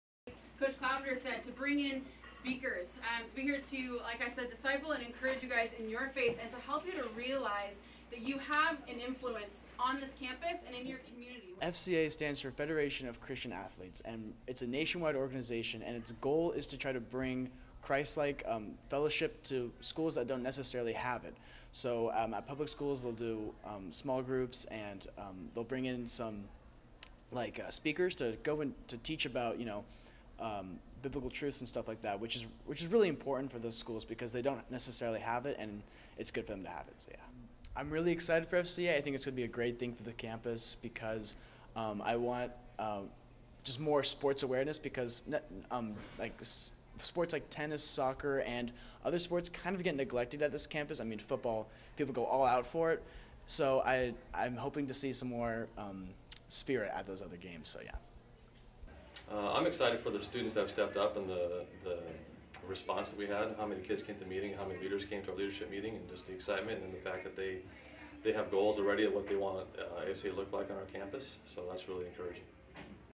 FCA Story
Fellowship of Christian Athletes begins meeting at Tr-City School